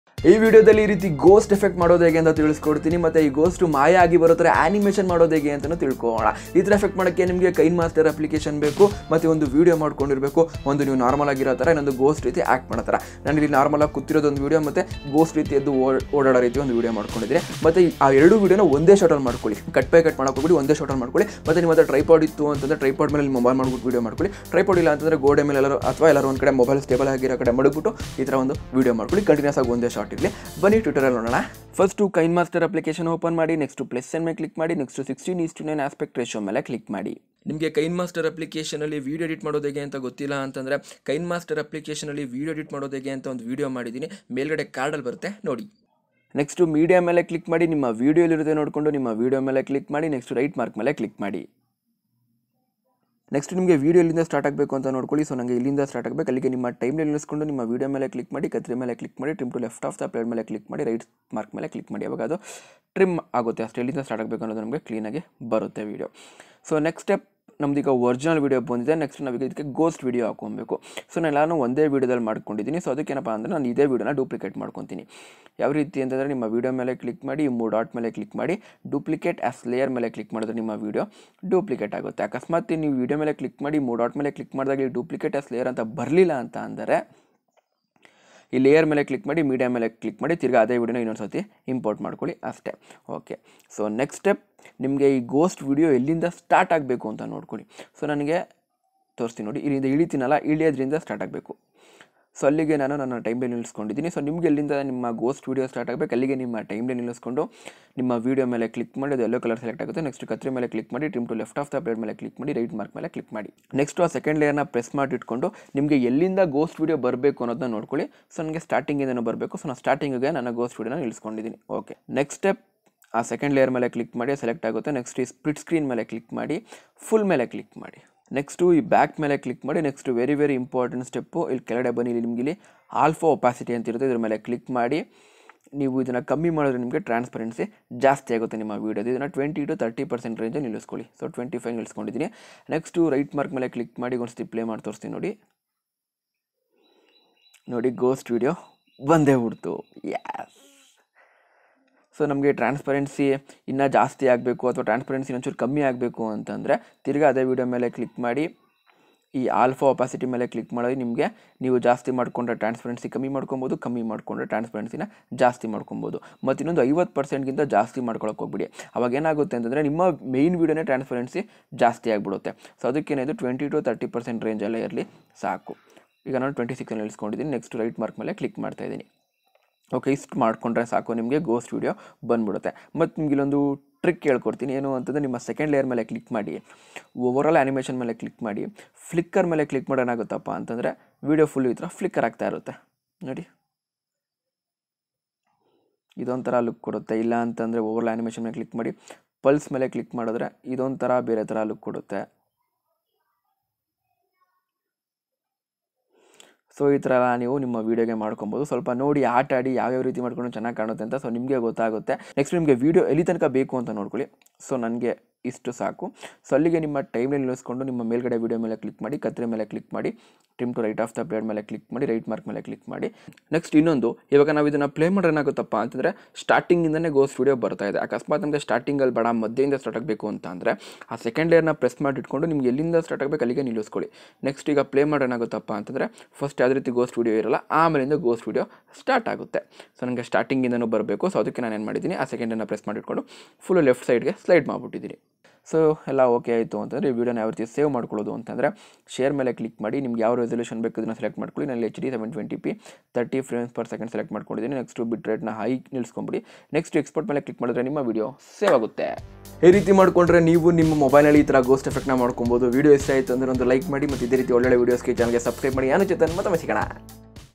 how to make ghost effect in mobile kannada | video editing kinemaster | film guru
In this video i will explain how to make ghost effect in mobile in kannada using kinemaster application.